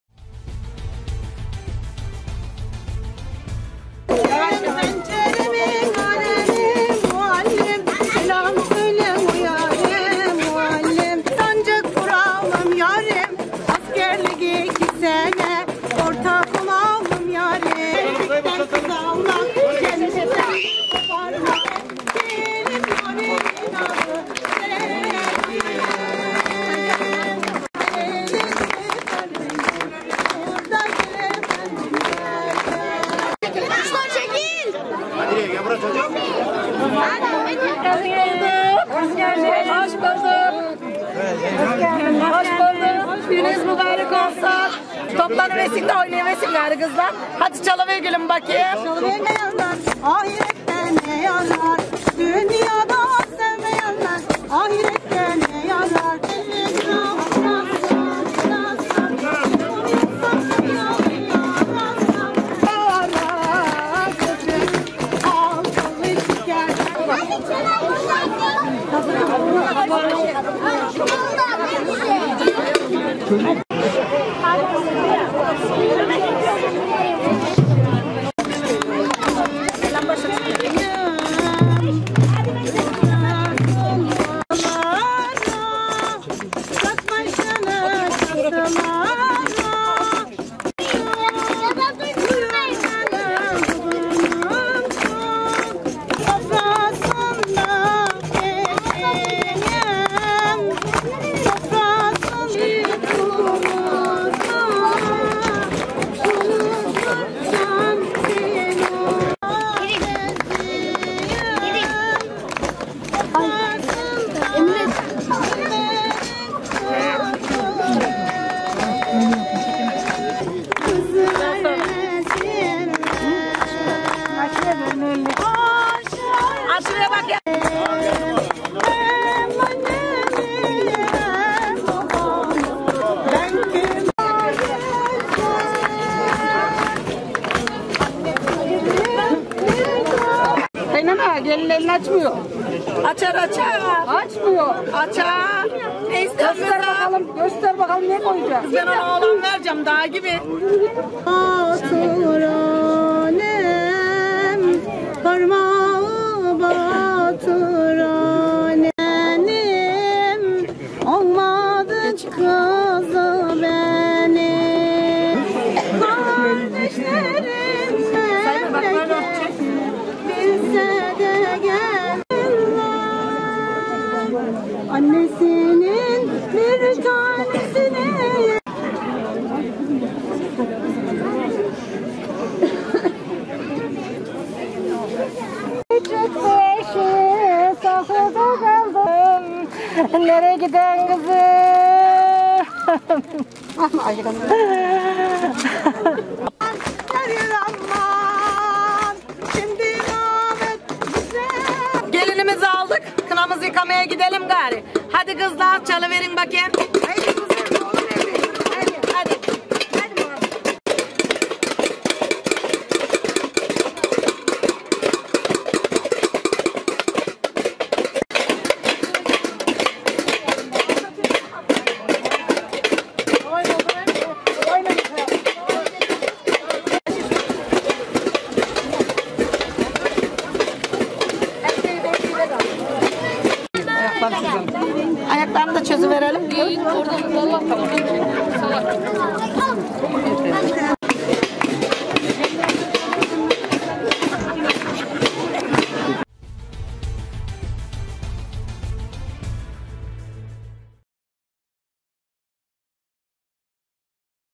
مصر العربية | احتفال "ليلة الحناء".. ثقافة يحتفظ بها الأتراك الرحل منذ ثلاثة قرون
رغم مرور ثلاثة قرون لايزال الأتراك الرحل الذين يطلق عليهم في تركيا اسم الـ "يوروك"، المقيمين بولاية "آيدن"(غرب)، يحتفظون بعاداتهم وثقافتهم المتبعة في الأعراس، لاسيما الاحتفال بـ "ليلة الحناء".